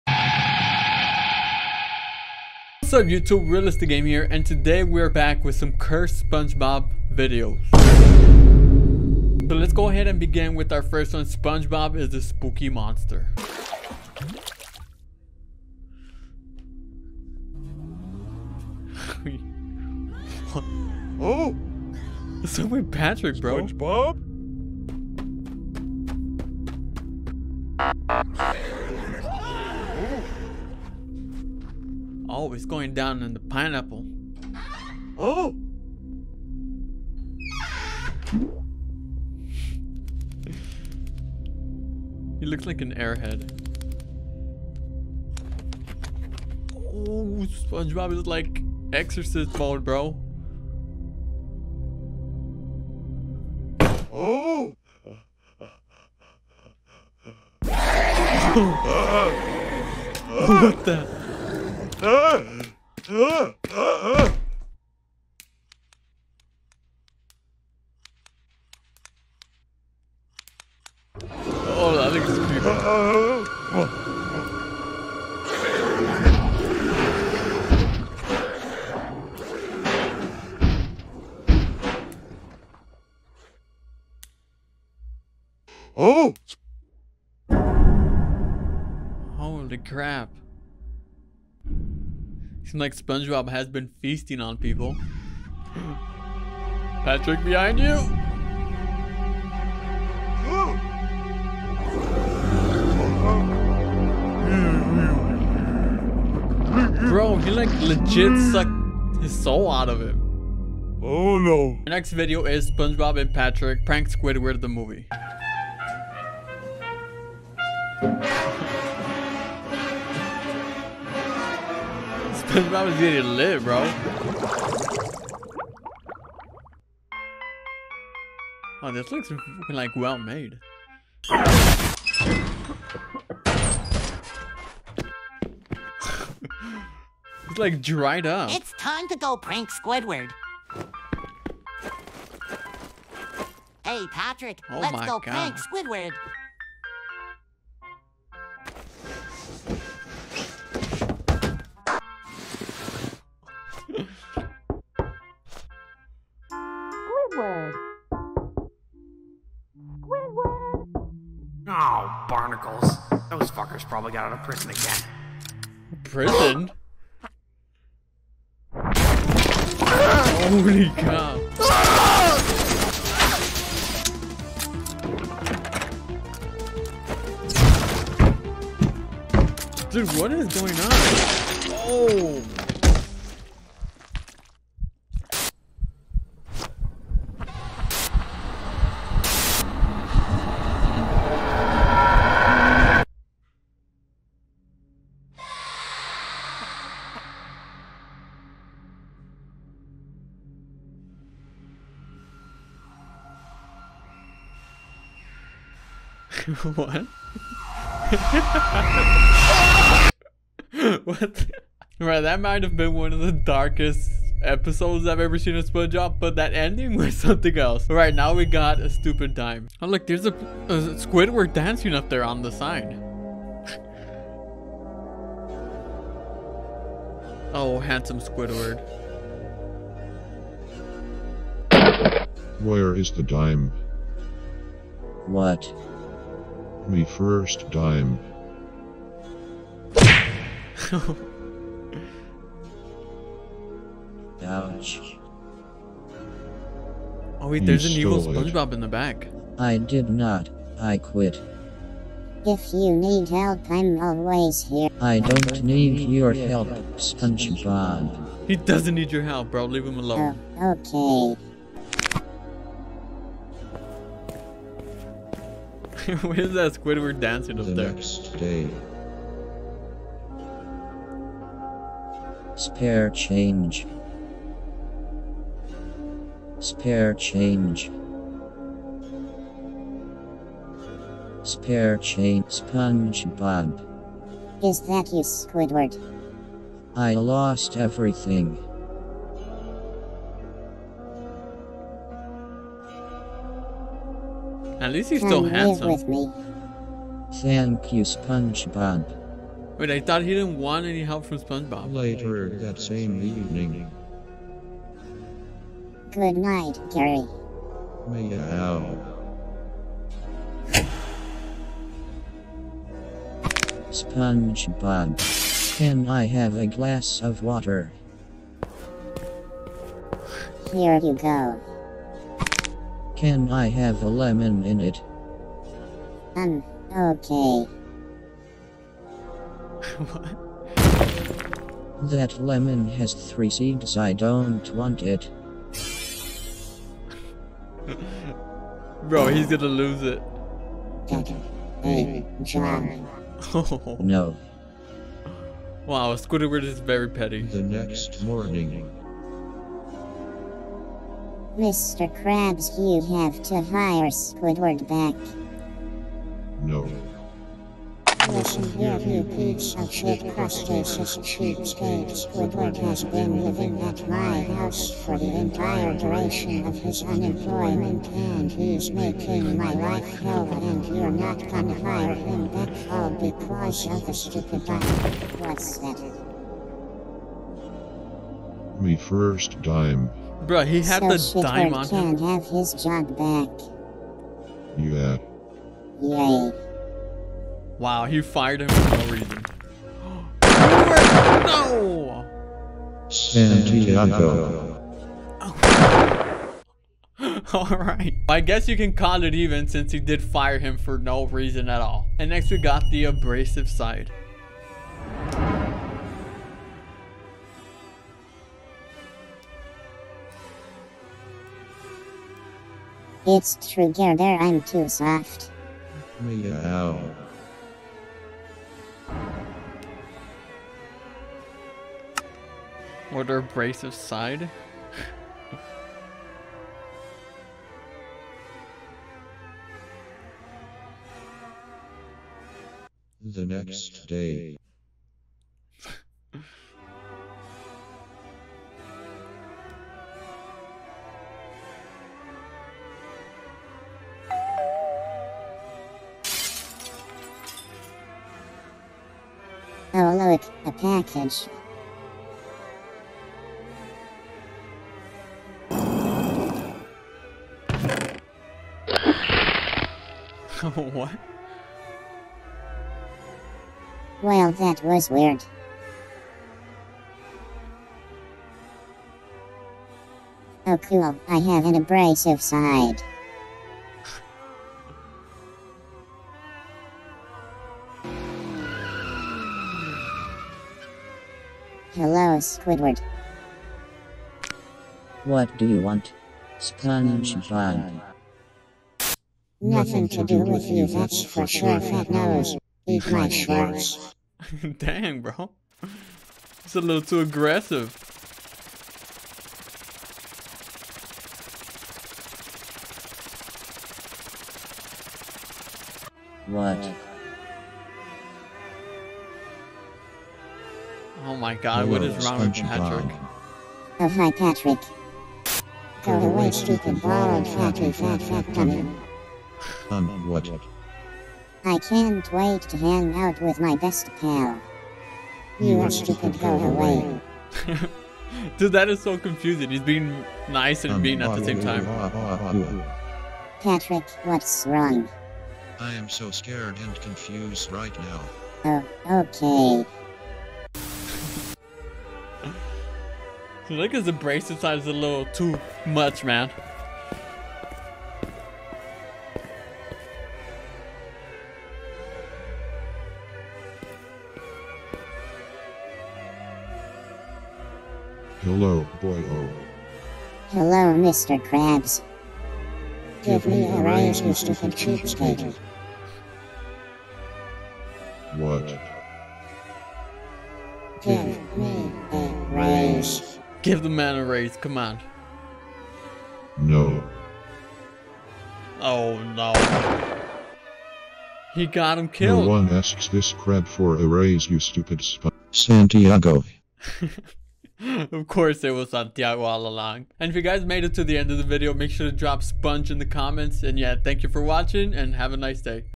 SCARY SPONGEBOB HORROR VIDEOS (spongebob.exe)
I dare you to watch these scary SpongeBob videos without hiding in the comments!